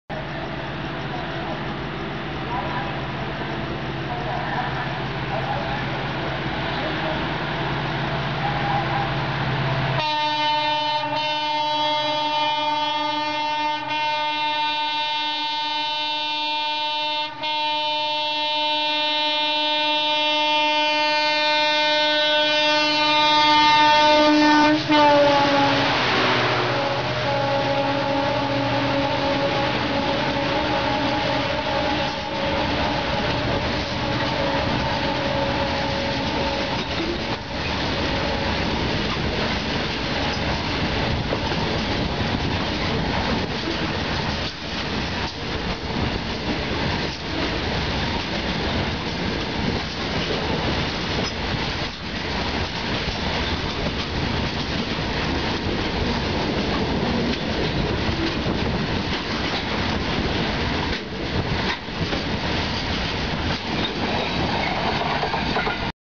SBI WDG4 BCNA Rake passes Etawah
SBI WDG4 along with BCNA Rake passes Etawah with BCNA Rake in tow.